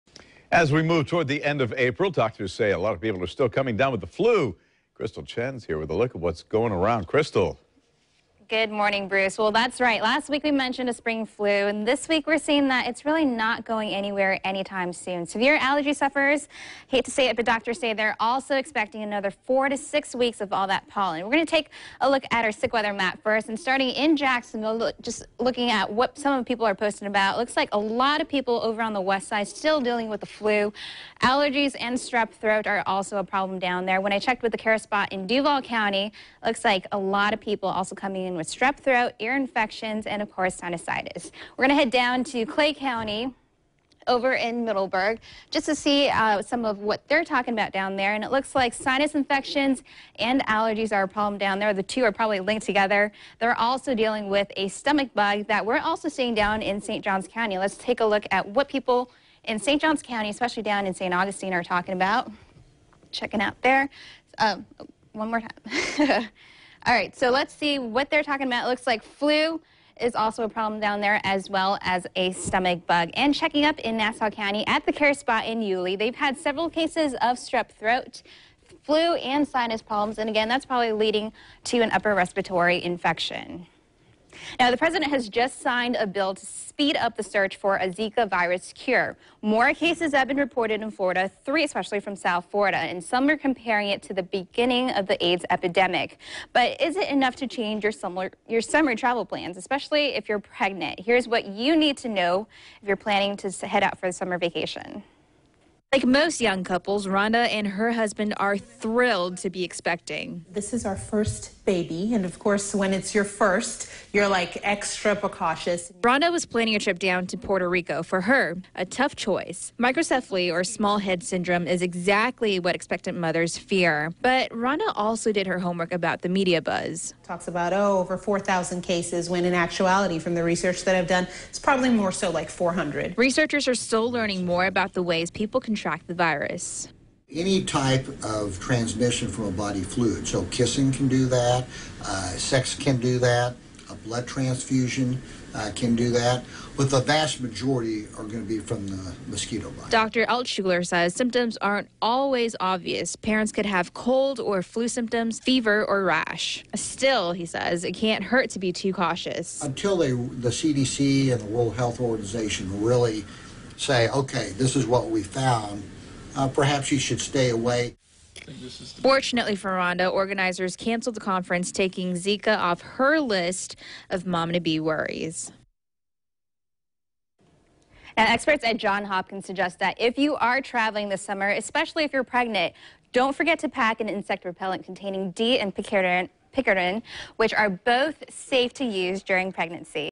What's Going Around - Zika traveling
The President has just signed a bill to speed up the search for a Zika virus cure.